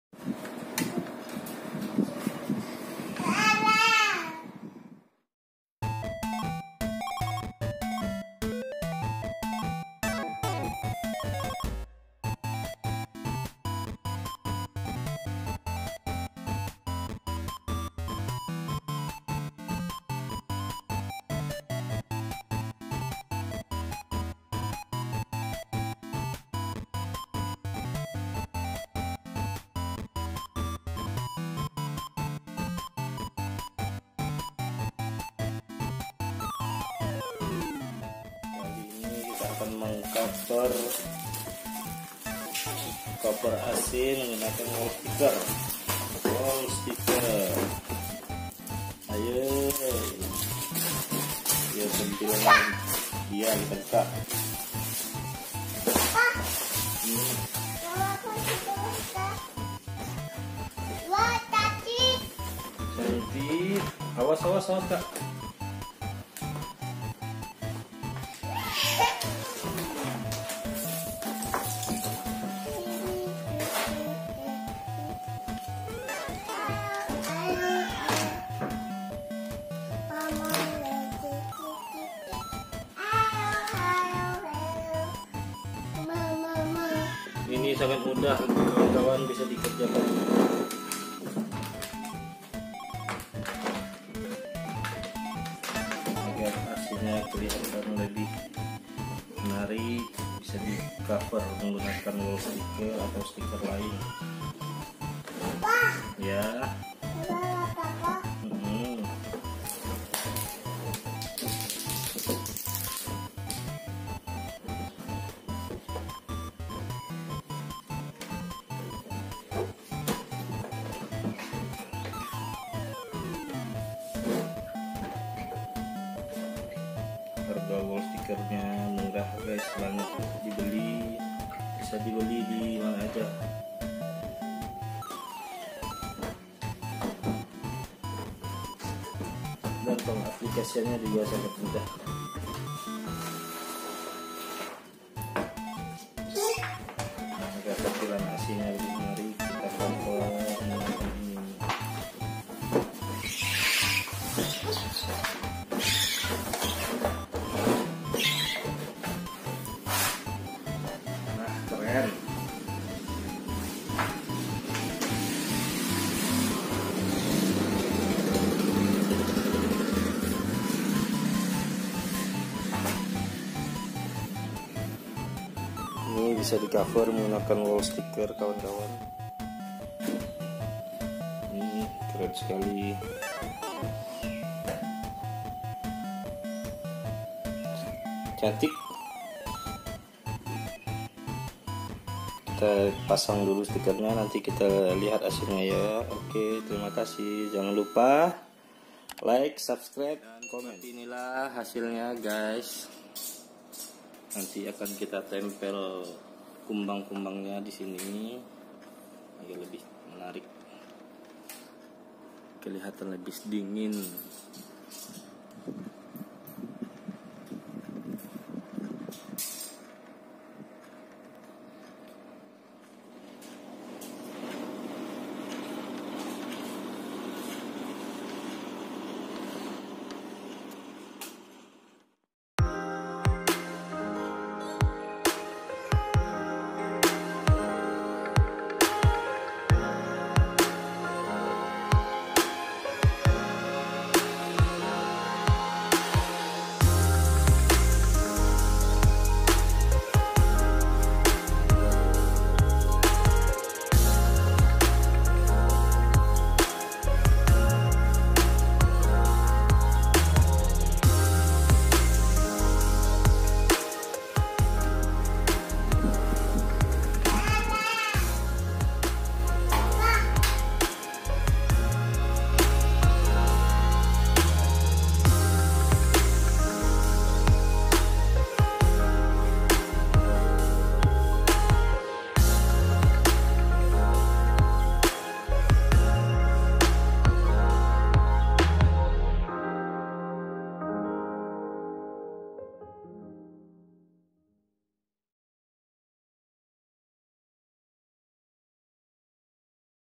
RESTORASI HANYA SEDIKIT SENTUHAN... AC JADUL JADI LEBIH KEREN | pasang stiker pada AC
Bismillah
Dikonten x ini saya mengaplikasikan wall sticker pada permukaan cover mesin AC...

Cara ini bisa di ikuti dirumah teman teman